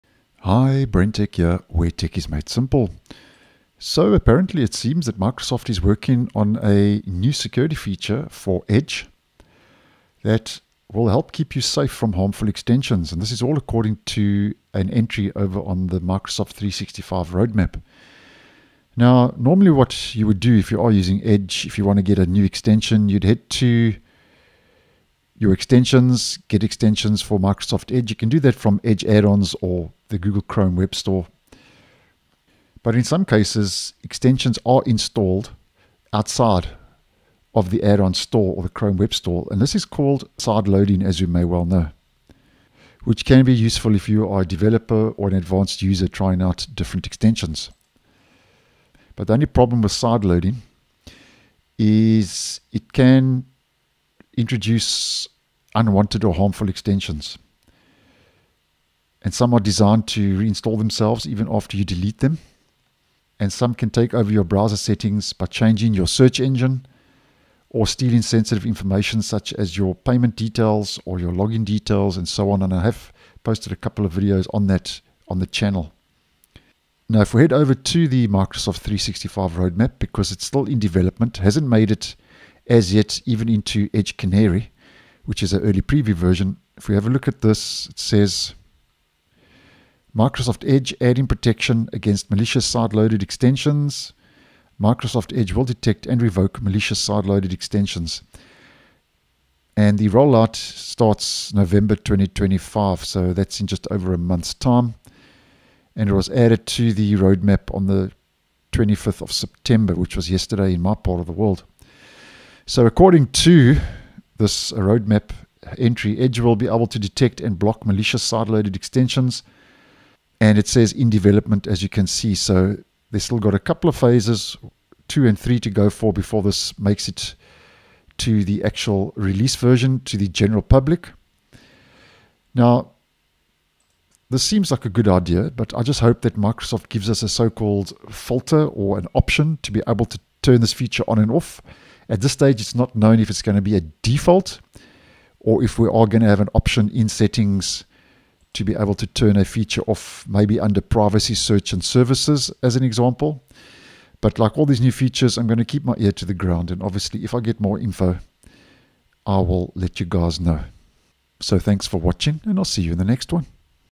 Microsoft Edge Will Soon Block Malicious Sideloaded Extensions! (Upcoming Feature)
The new feature means Edge will actively look for sideloaded extensions that are found to be dangerous